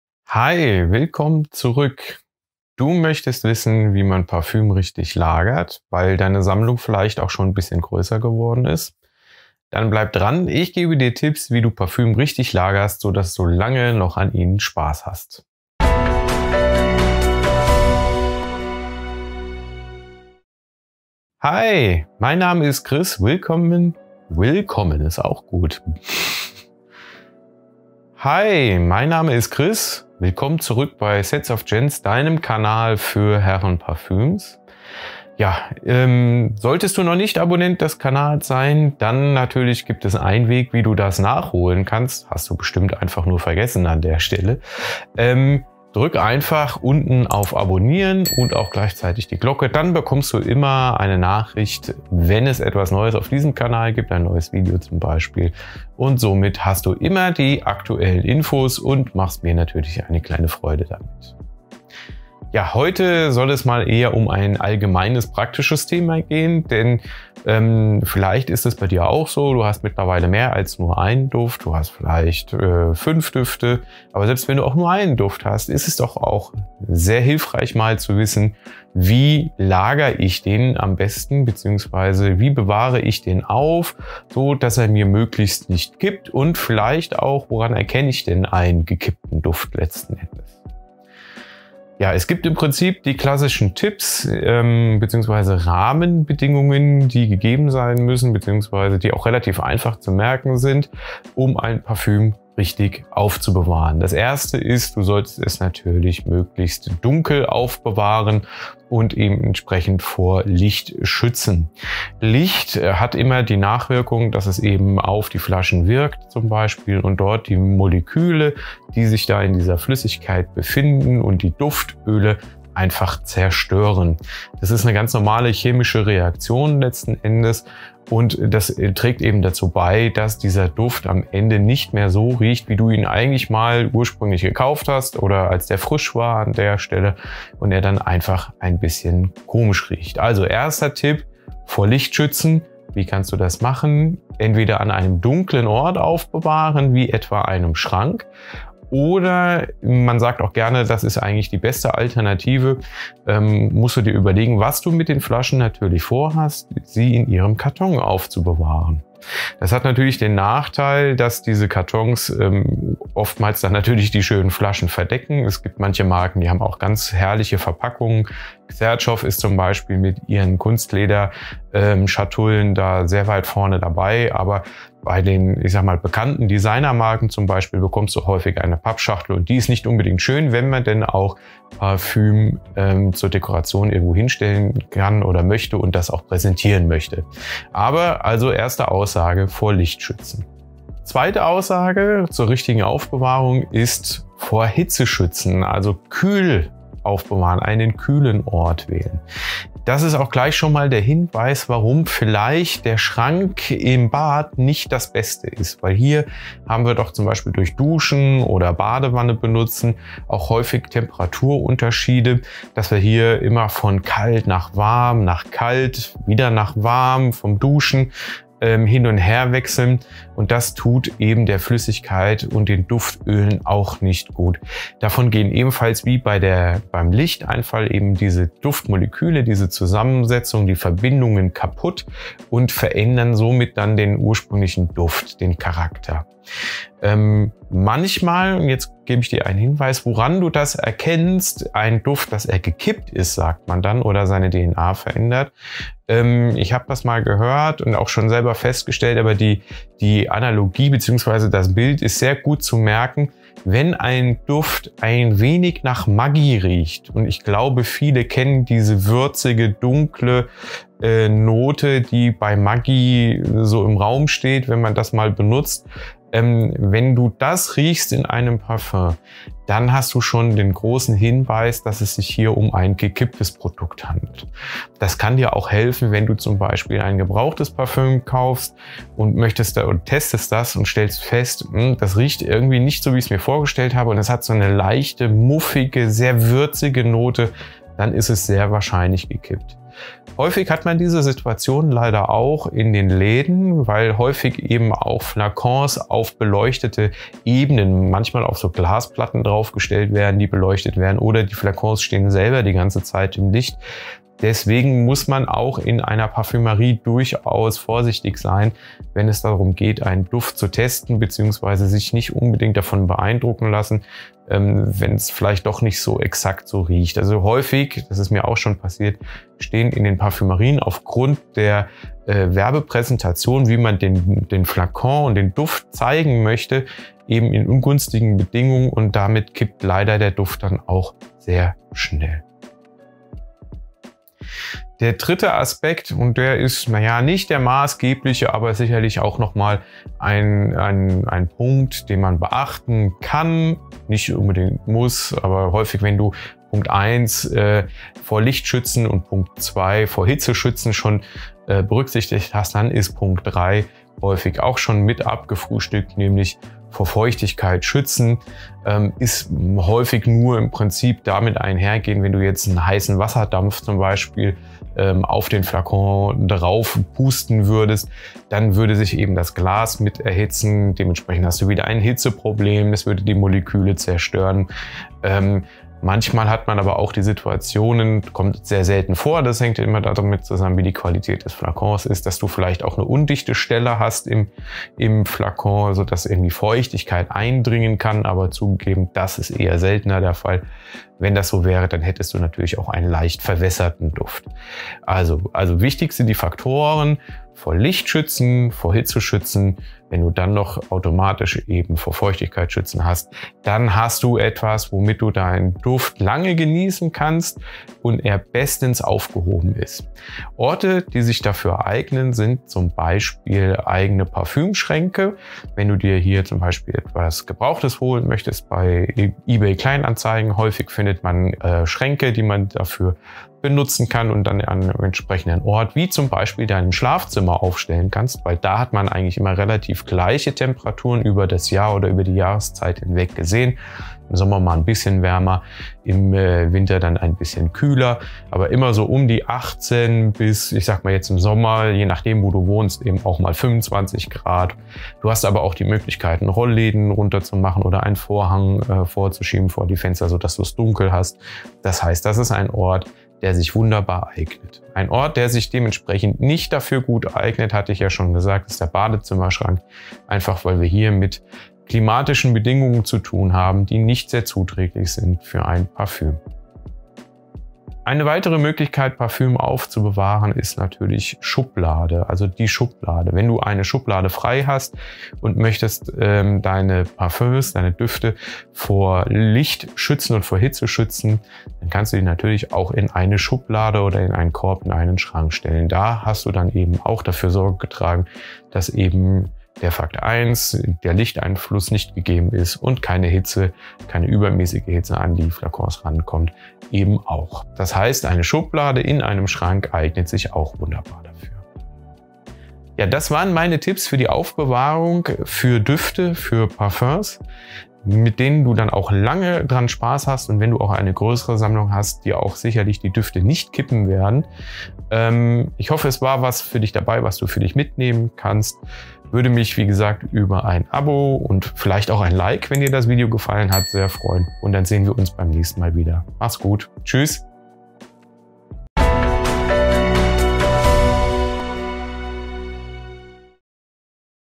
Parfum Tipp | Parfüm richtig aufbewahren | Wie ein Duft nicht kippt
In diesem Video gebe ich dir Tipps wie du einen Duft richtig aufbewahrst ohne das er kippt. Und ich erkläre dir, woran du einen gekippten Duft erkennst.

Alle vorgestellten Düfte sind von mir gekauft worden. Ich kann daher meine freie eigene Meinung zu den Düften euch berichten.